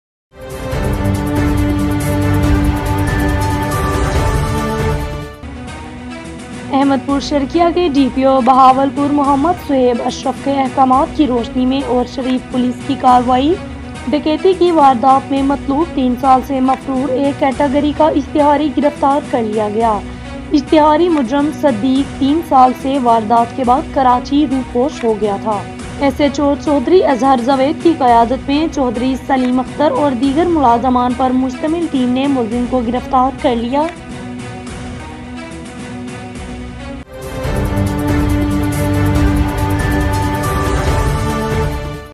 احمدپور شرقیہ۔ ڈی پی او بہاول پور محمد صہیب اشرف کے احکامات کی روشنی میں اوچ شریف پولیس کی کارروائی
Great news HD TV 21st July 2020 🇵🇰📺🇵🇰📺👈👈👈